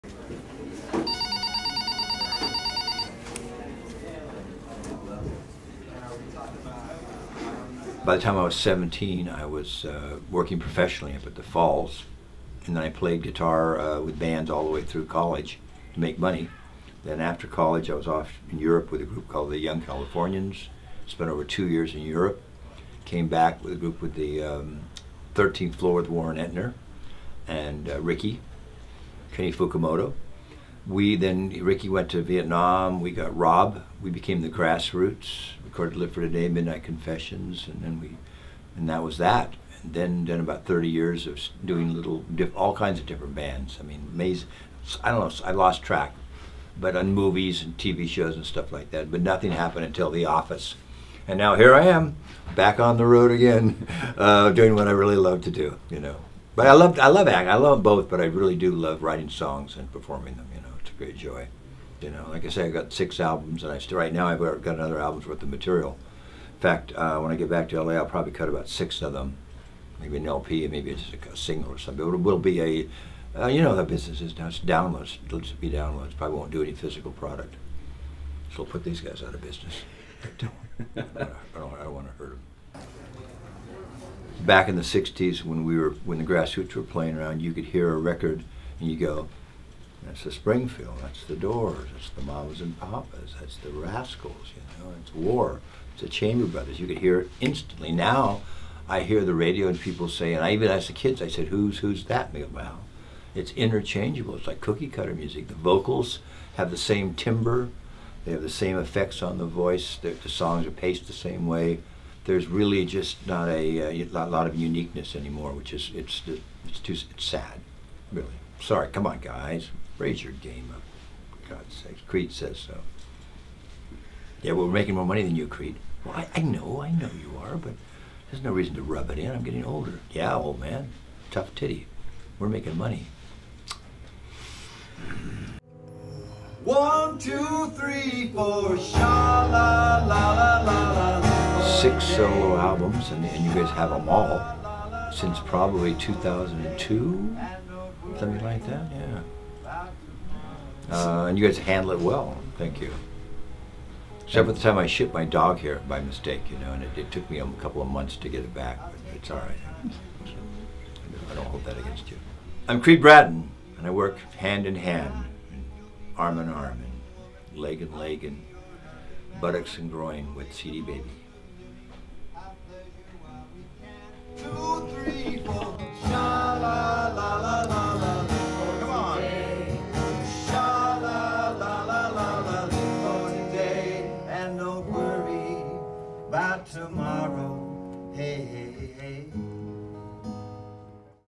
Creed Bratton at the CD Baby Office
Long time CD Baby musician, member of The Grass Roots, and actor Creed Bratton (The Office) stopped by our Office to say hello.